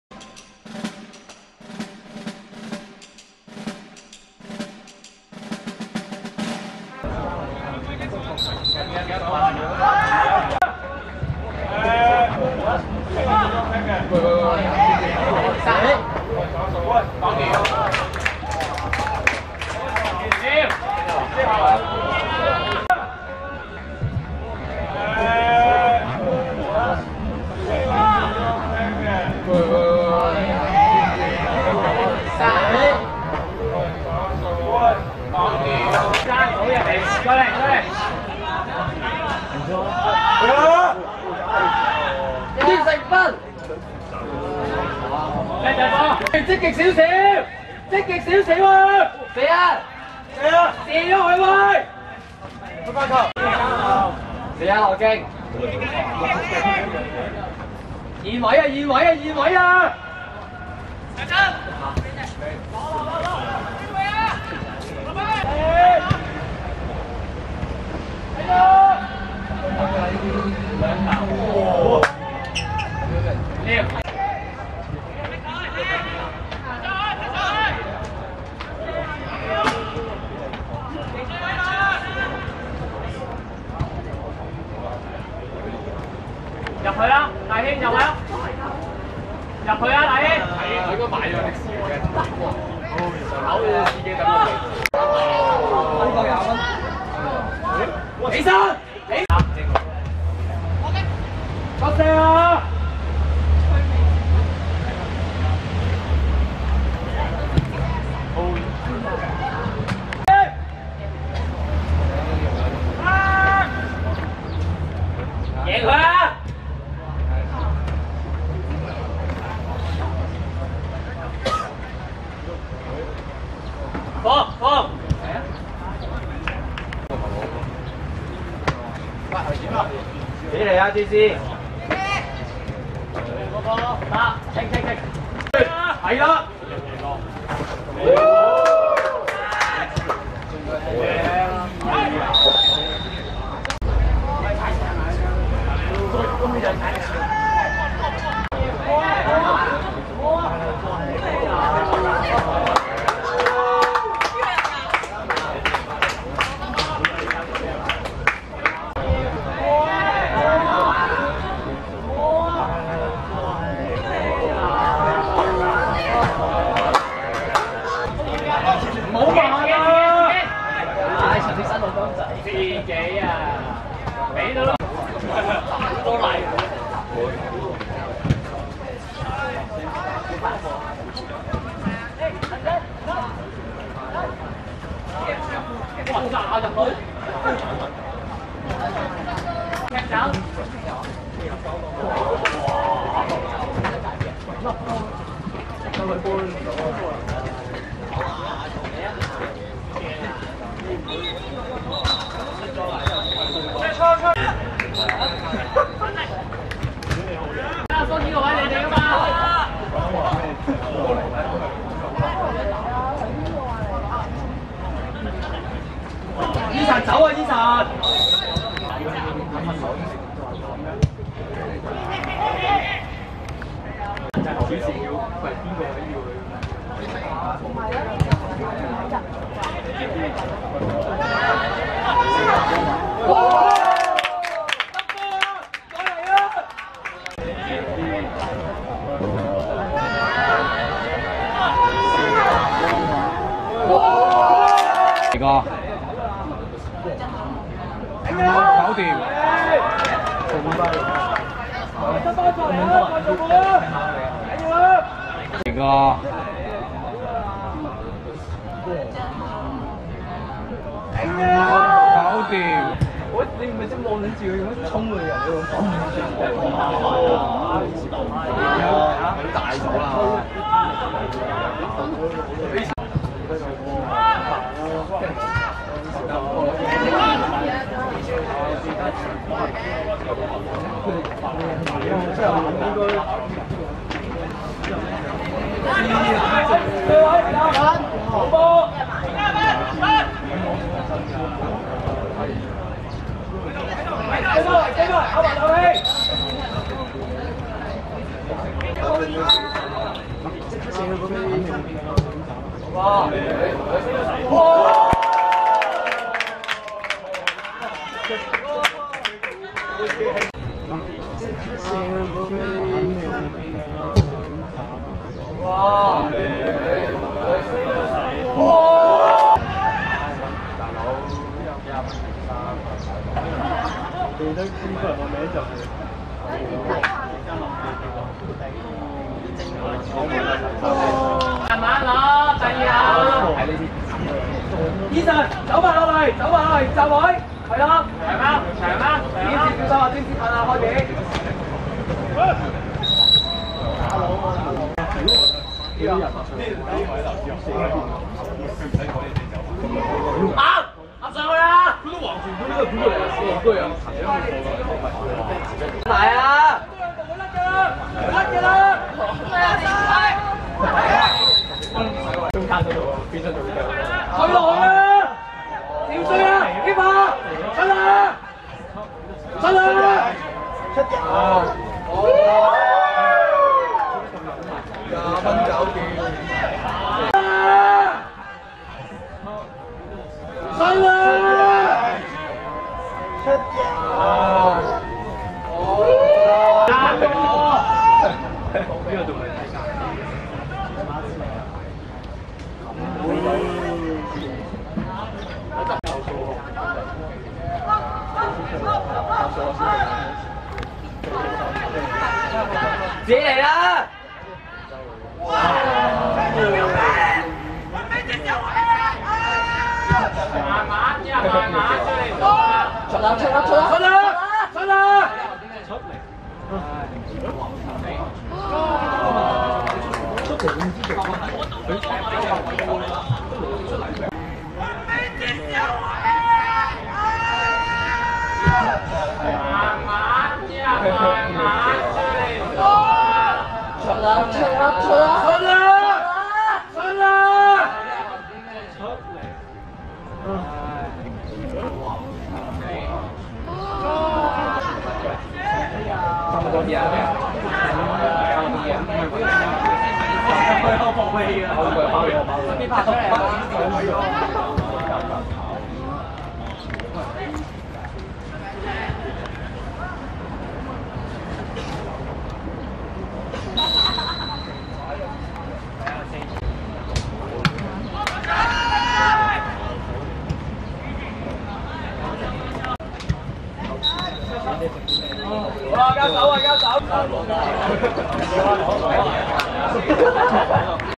伊中舊生會vs基元(2016.9.24.元朗學界足球甲組)精華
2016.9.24.天業路仿真草塲 10:30 伊中舊生會(藍) 5 : 2 基元(黑)